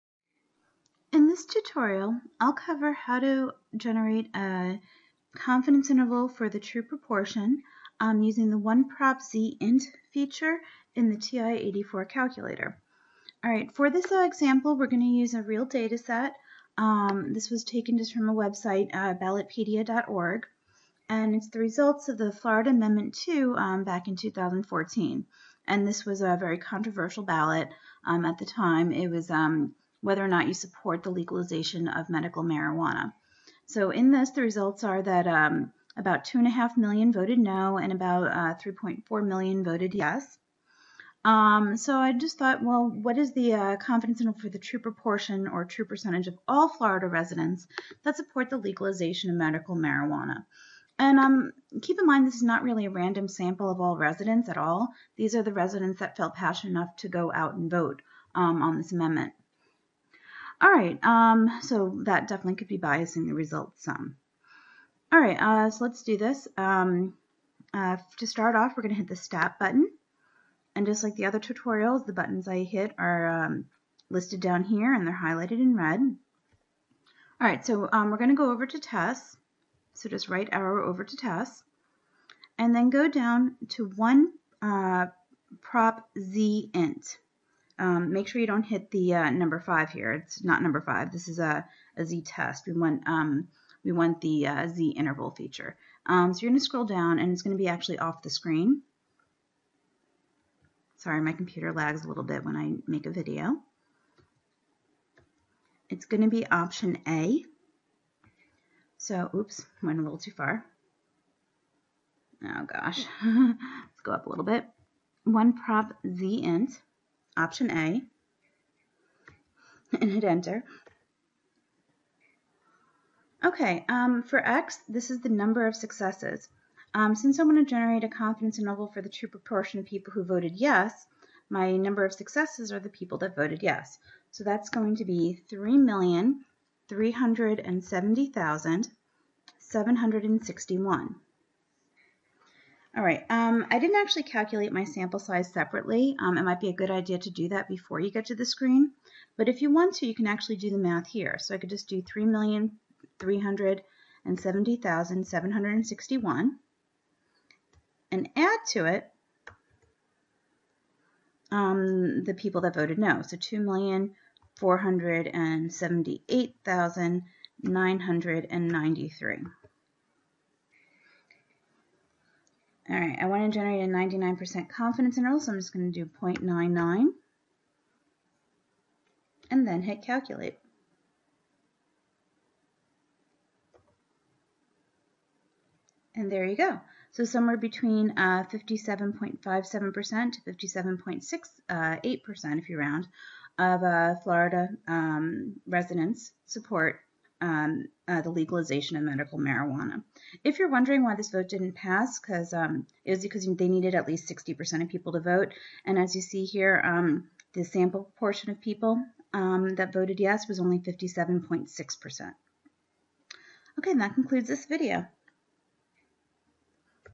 TI-84 Tutorial - CI for p (1-prop zint)
Tutorial on how to use the TI-84 calculator to generate a CI for p (the population proportion or percentage).